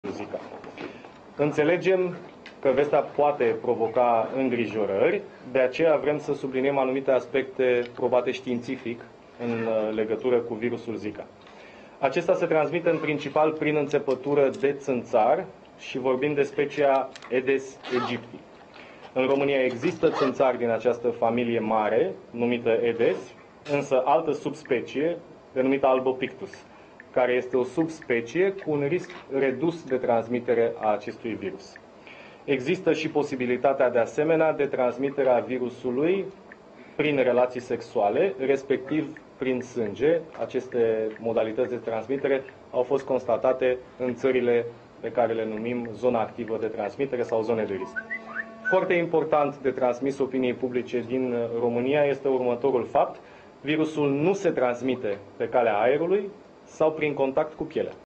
Conferință de presă organizată de purtătorul de cuvânt al Ministerului Sănătății...
Laurențiu Colintineanu: Virusul Zica nu se transmite pe calea aerului sau prin contact cu pielea!
Titlul evenimentului: Conferință de presă organizată de purtătorul de cuv&#226;nt al Ministerului Sănătății, Laurențiu Colintineanu, și conf. dr. Gabriel Popescu, specialist &#238;n boli infecțioase, consilier al Ministrului Sănătății